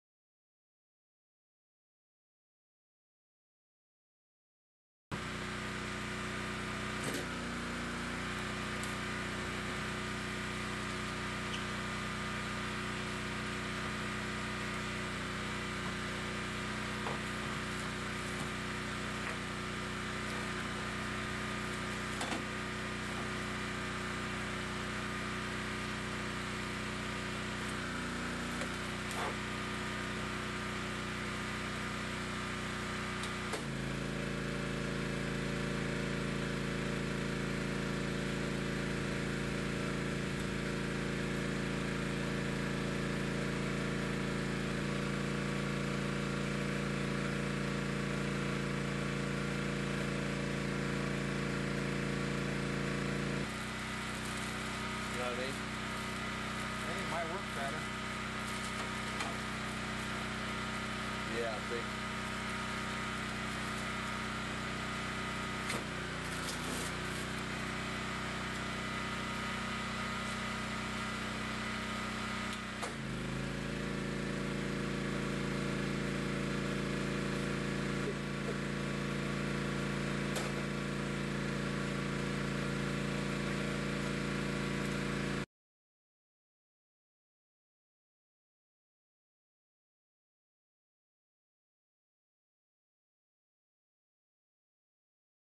Shrink Wrapping with L-Bar
Shrink wrapping with an L-Bar sealer and shrink tunnel speeds up the process.